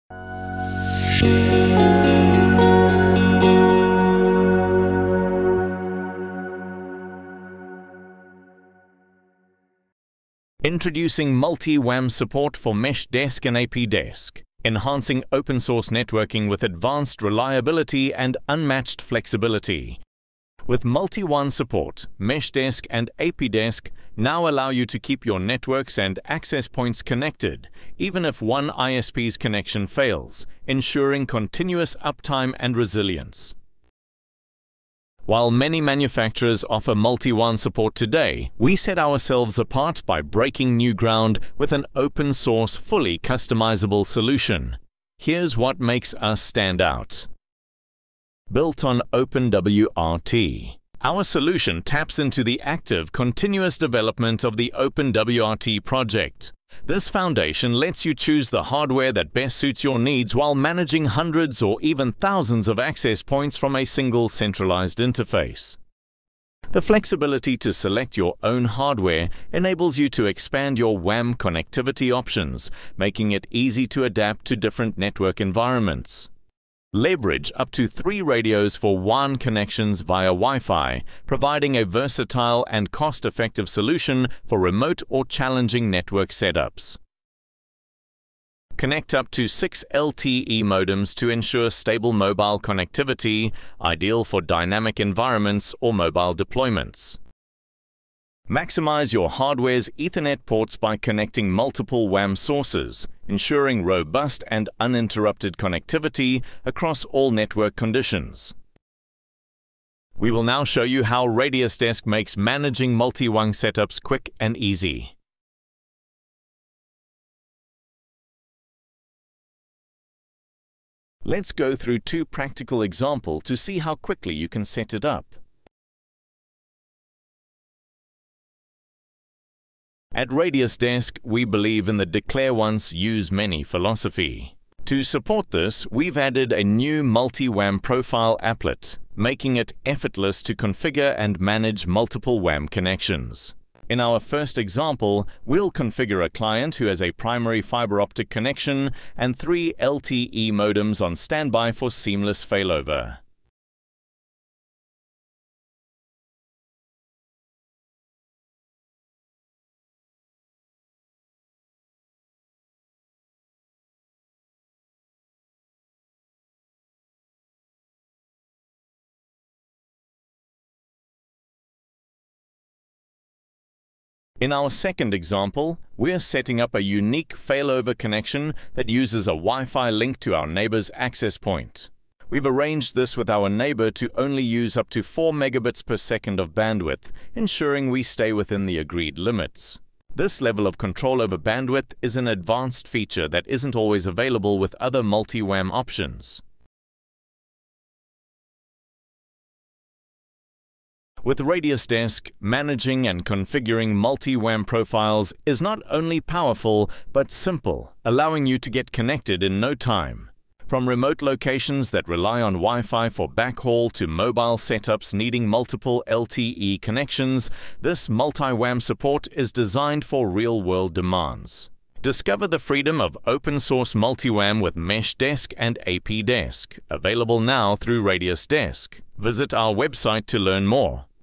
Simplify MultiWAN Management: RADIUSdesk Demo & Real-World Examples
Discover How Easy MultiWAN Management Can Be with RADIUSdesk!

In this video, we showcase how RADIUSdesk simplifies MultiWAN configurations using real-world examples. We highlight the powerful yet user-friendly MultiWAN profile applet that makes setting up and managing multiple WAN connections a breeze.

What's Covered:

    A quick overview of RADIUSdesk’s "declare once, use many" philosophy
    Example 1: Configuring a primary fiber-optic connection with LTE modems on standby for failover
    Example 2: Setting up a failover with a WiFi connection to a neighbor’s access point, including bandwidth control

With RADIUSdesk, you gain advanced features like precise bandwidth management, ensuring optimal performance and maintaining friendly agreements.

Watch now and see how RADIUSdesk can transform your network management!

    Follow us for more updates and tutorials.